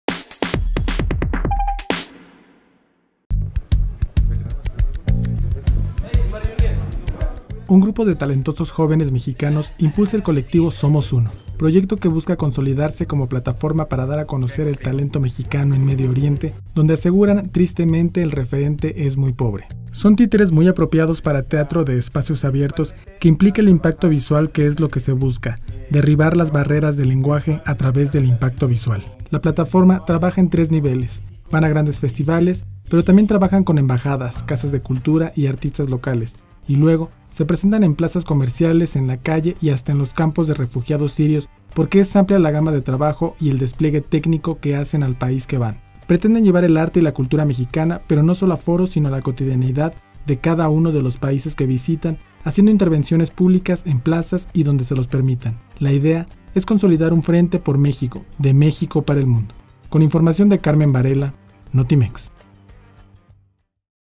Medio Oriente, un mundo por explorar para el arte y la cultura de México
Somos uno busca consolidarse como plataforma de vinculación de talentos mexicanos con esas tierras